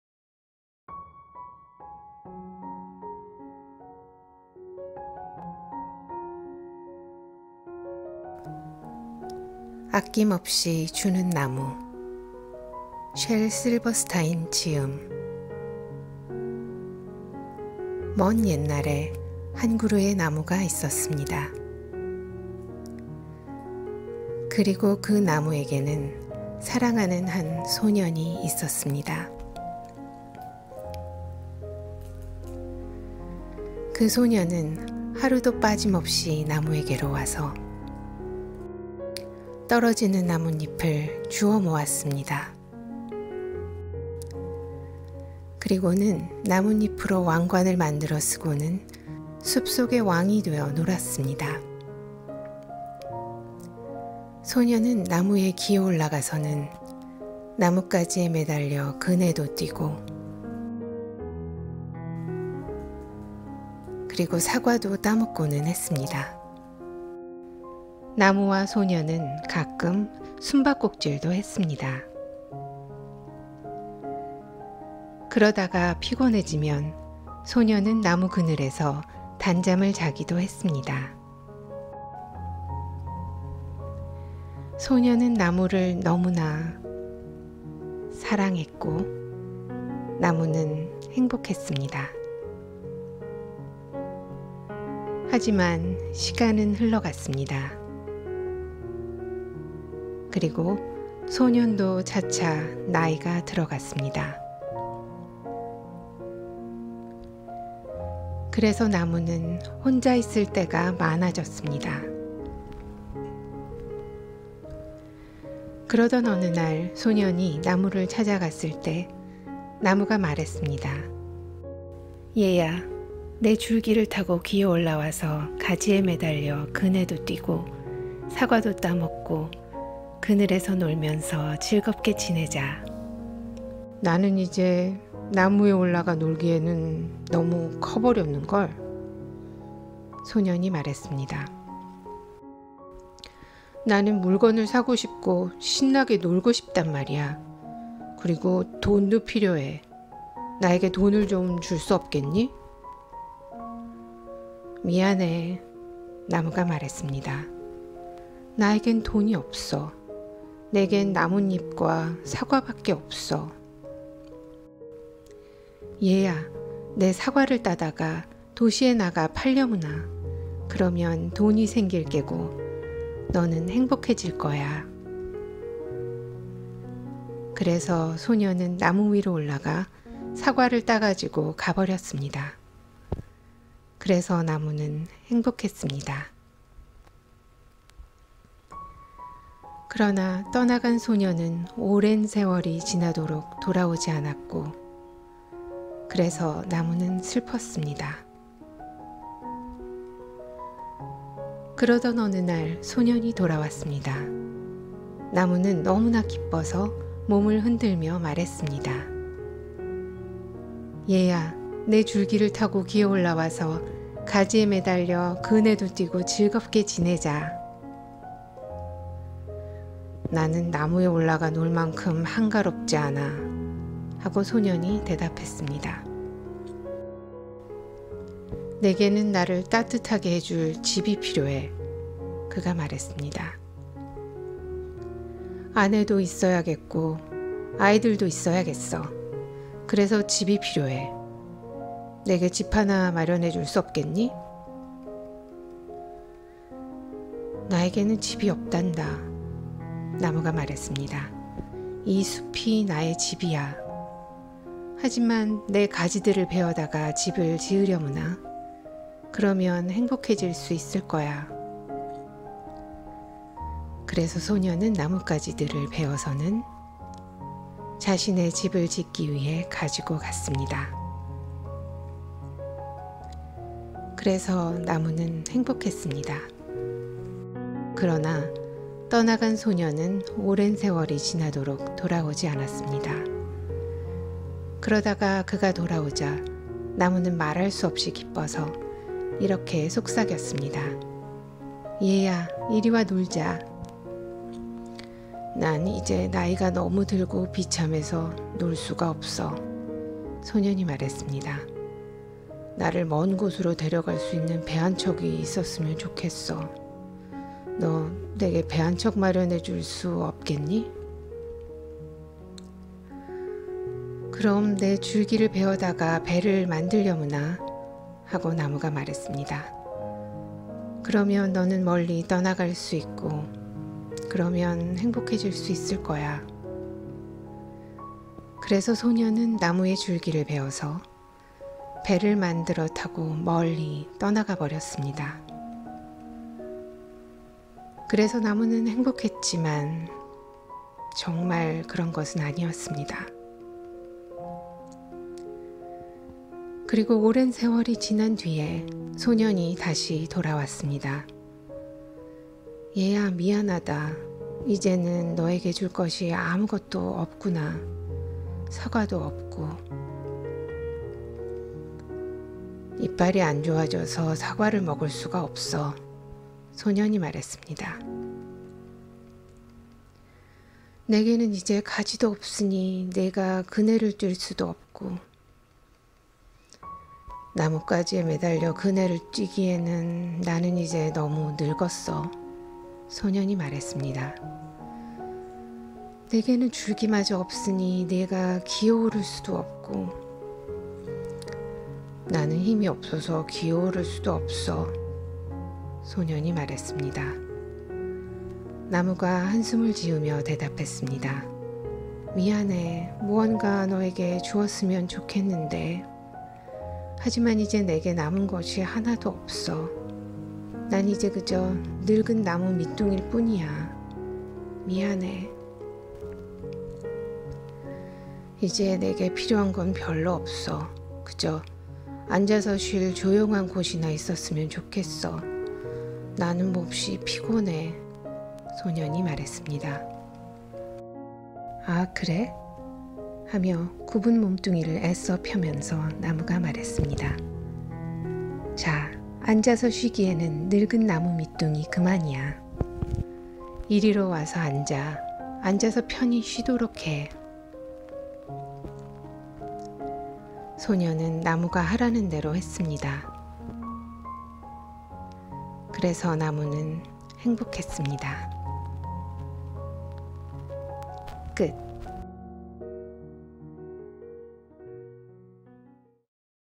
The Giving Tree / 아낌없이 주는 나무 / Story Read Aloud with English & Korean Texts
The Giving Tree by Shel Silverstein

아낌없이 주는 나무 - 셸 실버스타인 지음
                                      이다미/강민경* 옮김
* 부분적으로 출판본과 다르게 번역했음을 알려드립니다.

2:53 오타 Typo "생일 게고" should be  "생길 게고"